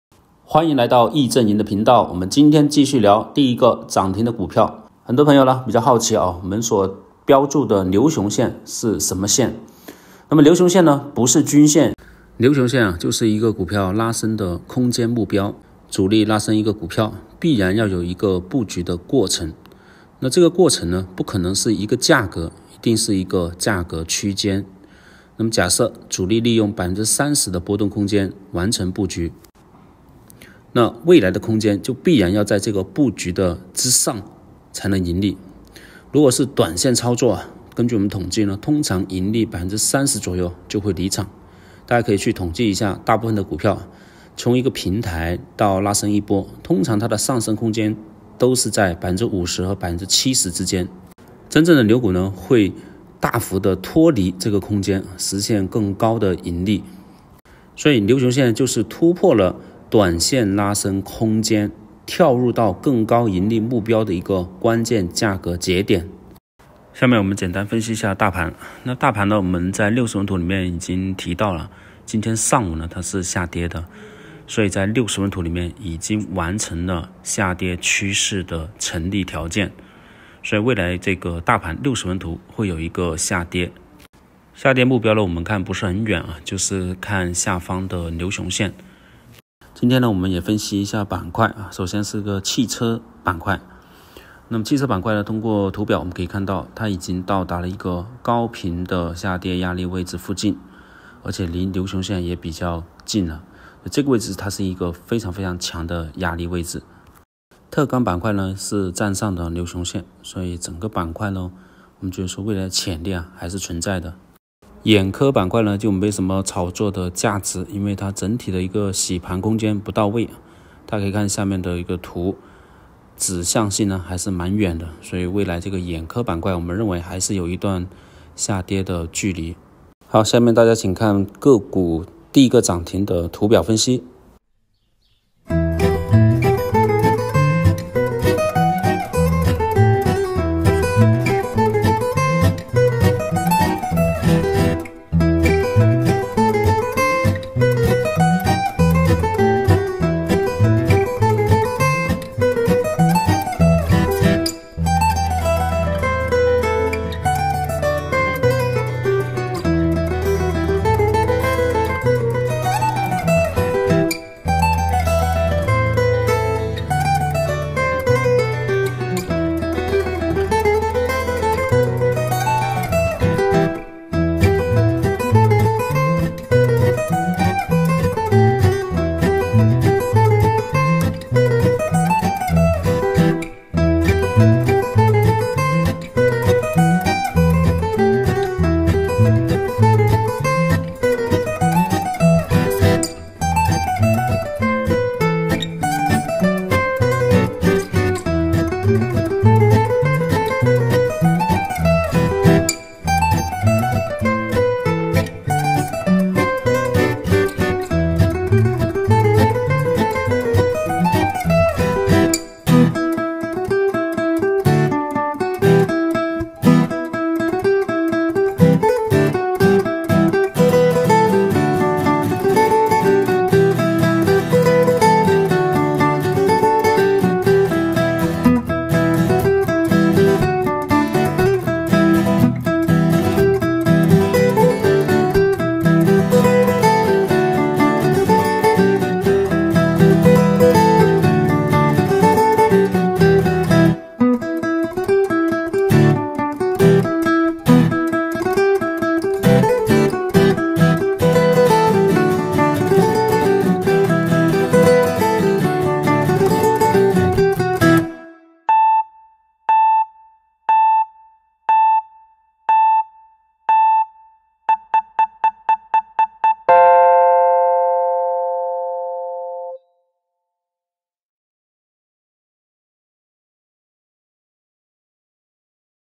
面壁者股民，我是你的破壁人，你的努力市场根本不在乎！
面壁者股民，我是你的破壁人，你的努力市场根本不在乎！对于技术指标，主力根本不在乎。